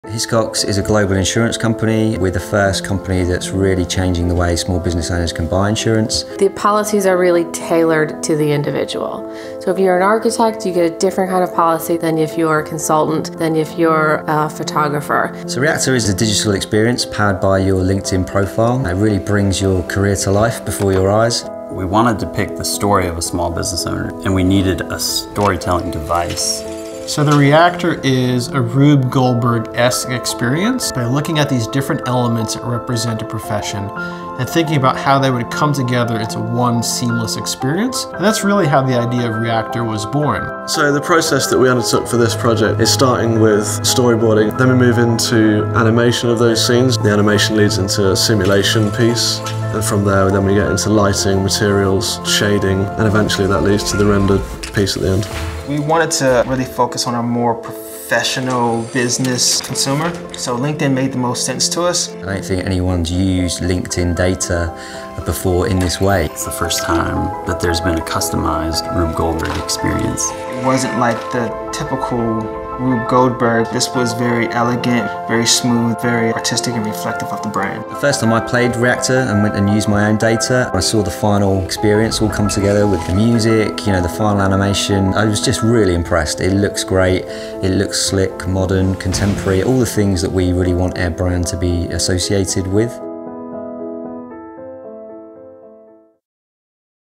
Behind the Scenes of Creating the Hiscox Reactor
The Hiscox Reactor is an online experience that takes users on a personalized virtual career journey and shows how their past actions can lead to future success powered by the details of their LinkedIn profile. The Hiscox Small Business Insurance team worked with the award winning Media Monks to bring this experience to life. This video shows a behind the scenes look at the making of the Hiscox Reactor. and see where your actions take you and your business.